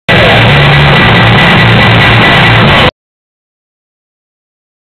FORD TOUGH BULL RIDING NYC Jan 2007 #2
one of the Bull riders at Madison Square Garde, January 2007